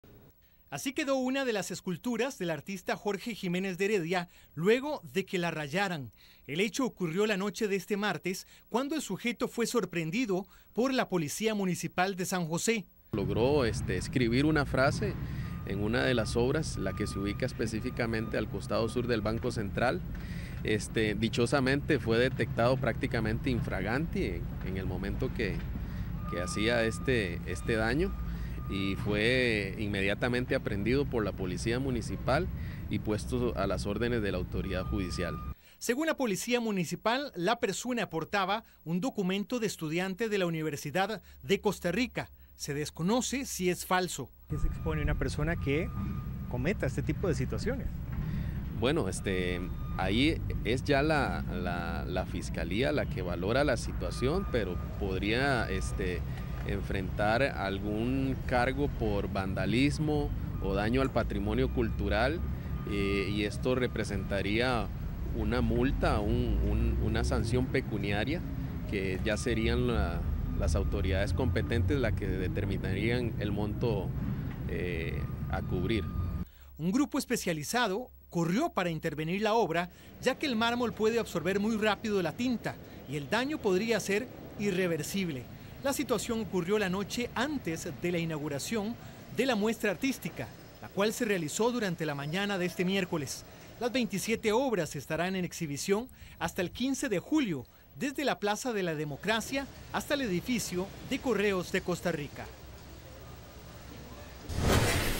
Vandalismo en una escultura de Jorge Jiménez Deredia
Un joven rayó una escultura de Jorge Jiménez Deredia y fue sorprendido por la policía.